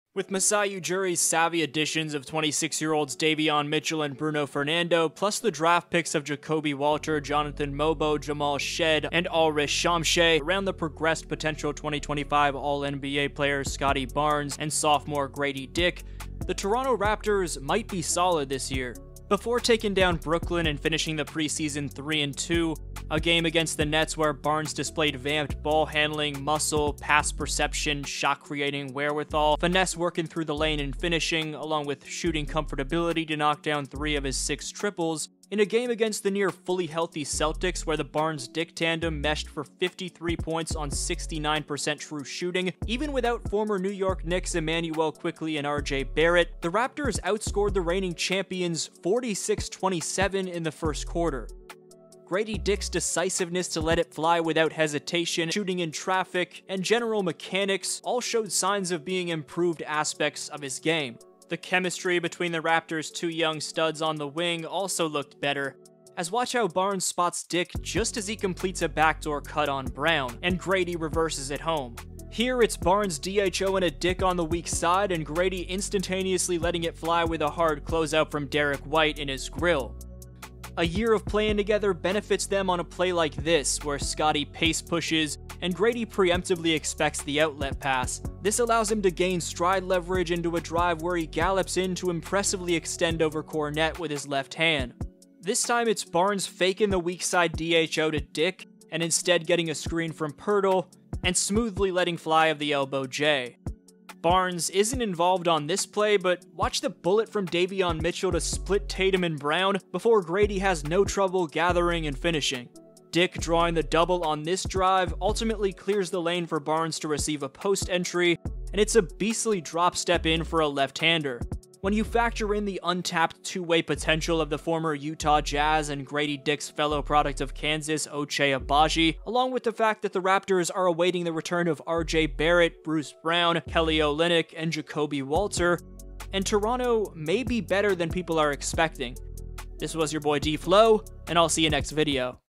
The Toronto Raptors Might Be SOLID…
Around the evidently improved Scottie Barnes and Gradey Dick, the Raptors took steps in the right direction as a franchise in terms of their acquisitions. Mostly I’ve been impressed by the progression of the tandem on the wing that’s seeming like they can build around into the future. Here’s how Barnes and Dick have developed, the chemistry they’ve gained and more on why the Raptors may be better than people think.

Last Video: The Boston Celtics Mistake COULD Be Costly…

0:00, Intro
0:19, Scottie Barnes’ Development
0:52, Gradie Dick’s Development
1:01, Barnes And Dick’s Chemistry
2:01, Ochai Abaji And Toronto’s Injuries

Beats: Via LCS